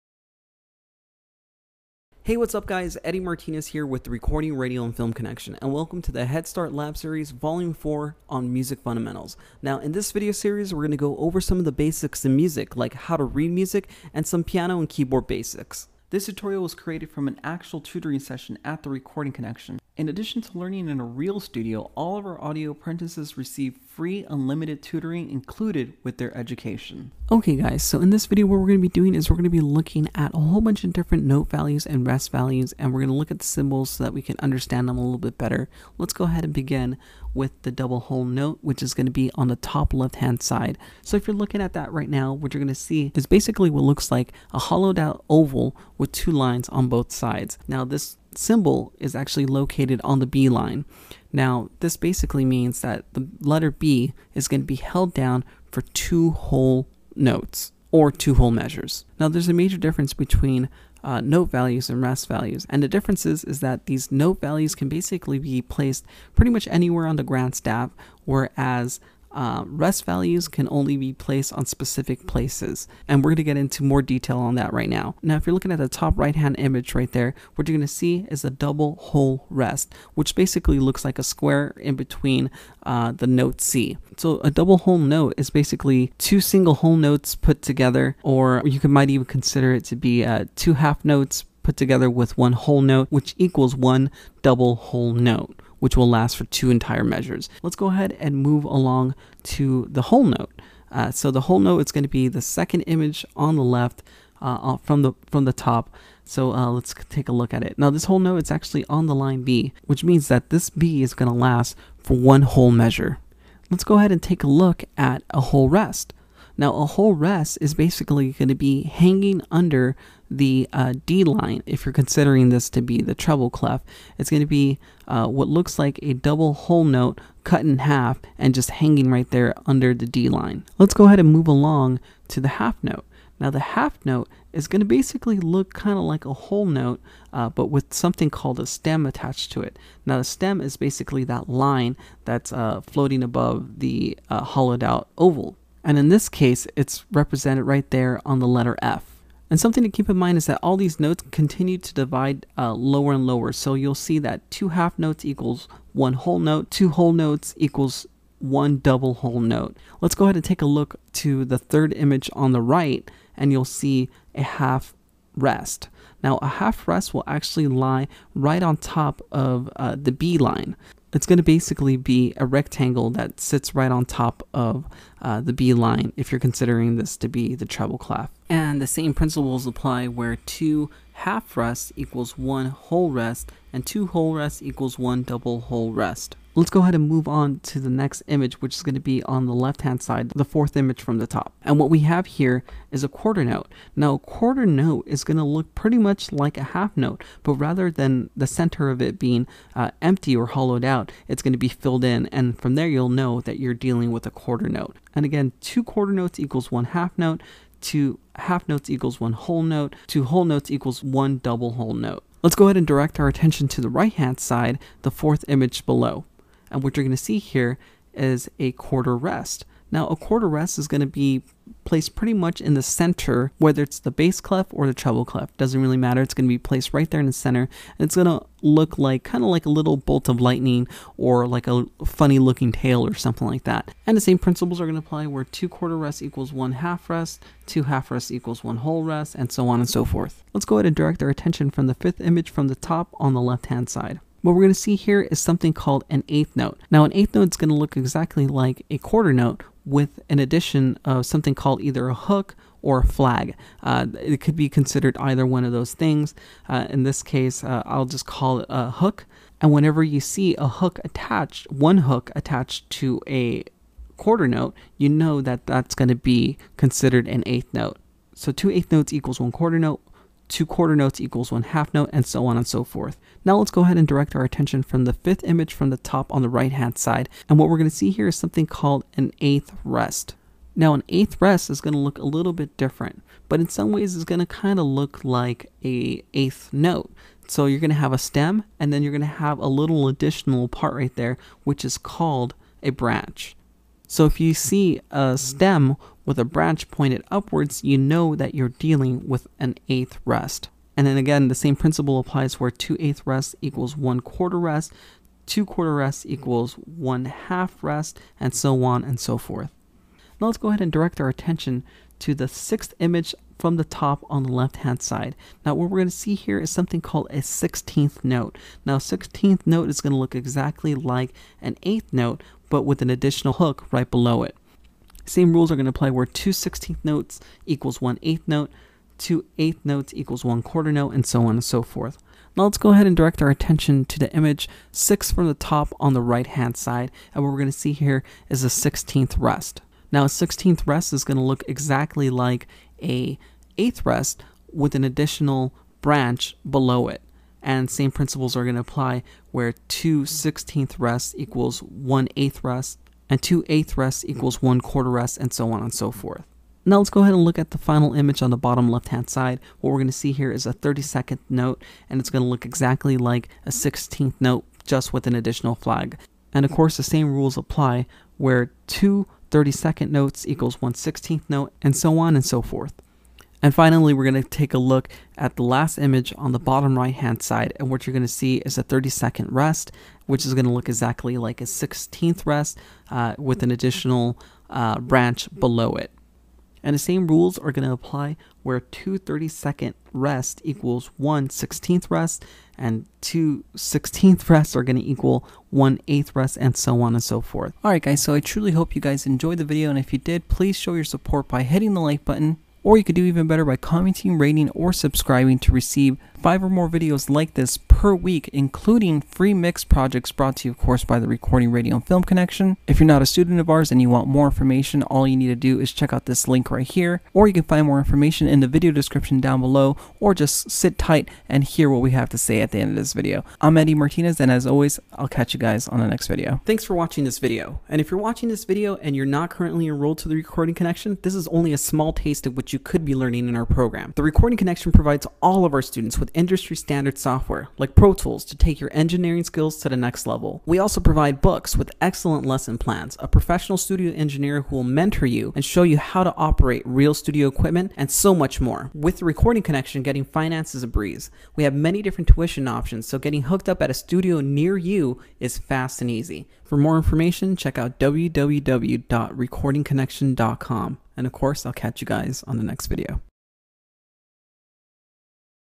HSLS 4 Note and Rest Values
Head Start Lab Series by the Recording Connection: Basic music appreciation, understanding and creation starts with knowledge of notes and rests and their respective values.